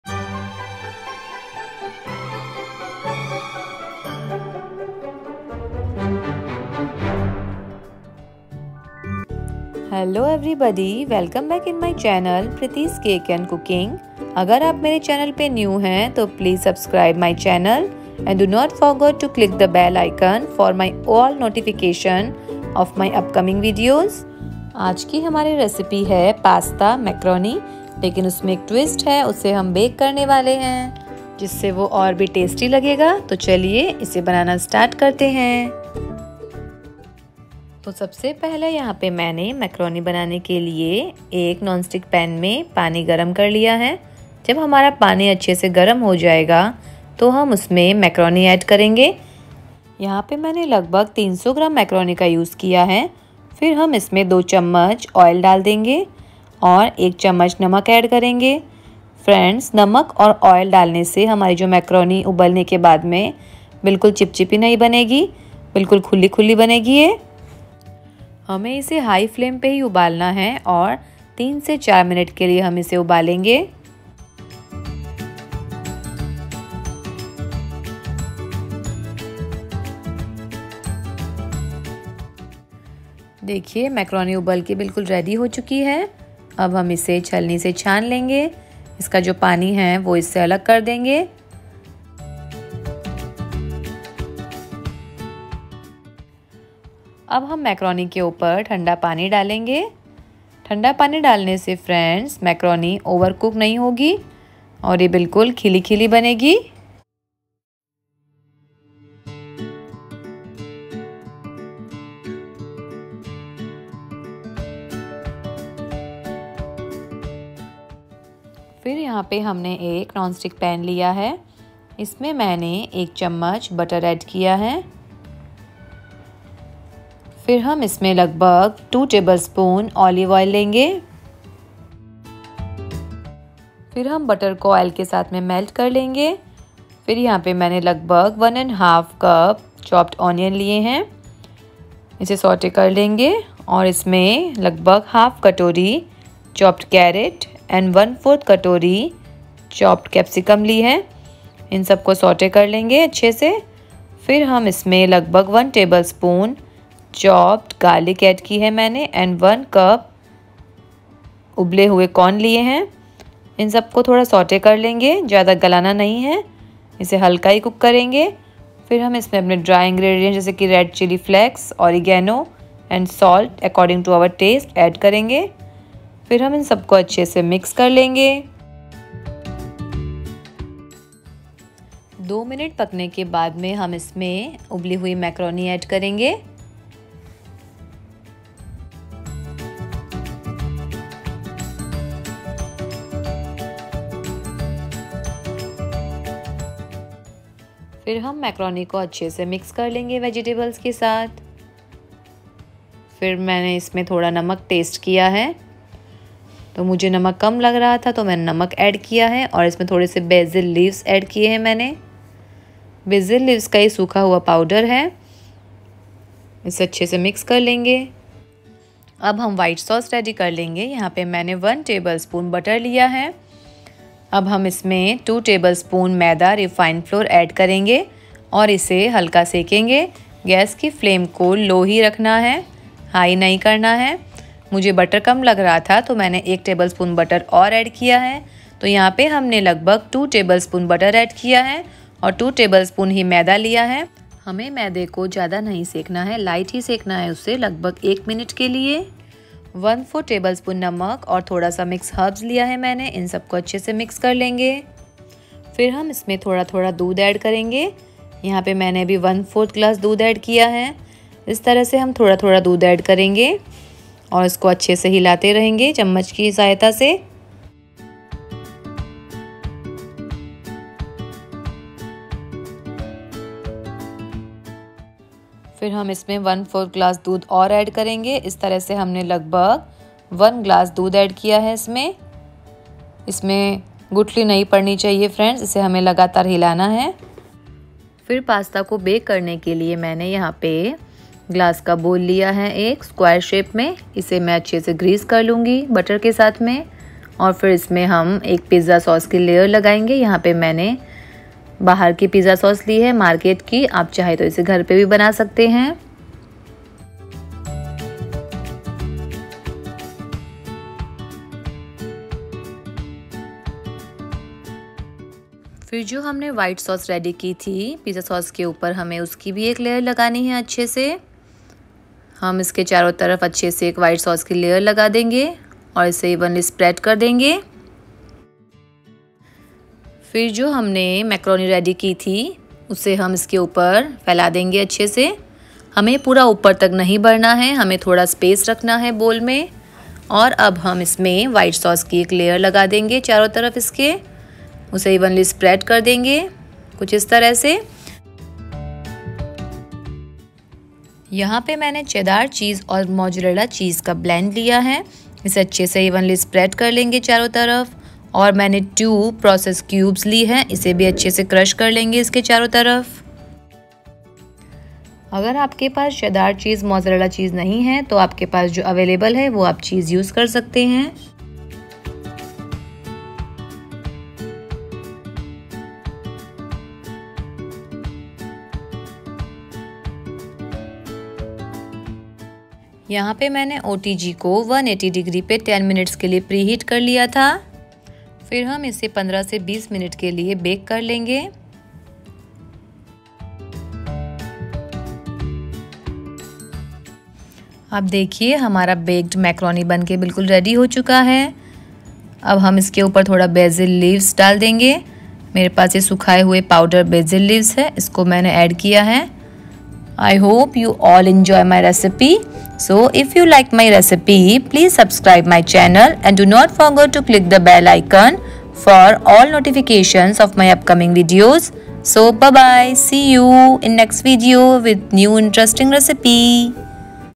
Baked Pasta Recipe। Macaroni ।pasta । white sauce Pasta
Ingredients-
White sauce-
2tbsp butter
2tbsp refined flour
1 glass milk
Salt
Mix herbs
Boiling Macaroni-
Water for boiling macaroni
Salt
2 tbsp oil
1 tbsp salt
Macaroni-
1tbsp butter
2 tbsp olive oil
11/2 cup chopped onion
1/2  cup chopped carat
1/4 cup chopped capsicum
1 tbsp garlic paste
1 cup boiled sweetcorn
Salt
Chilli flakes
Basil leaves
Mix herbs
For baking-
Glass bowl
Butter
Pizza sauce
White sauce
Cheddar cheese
Mozzarella cheese
Process cheese

Thank u.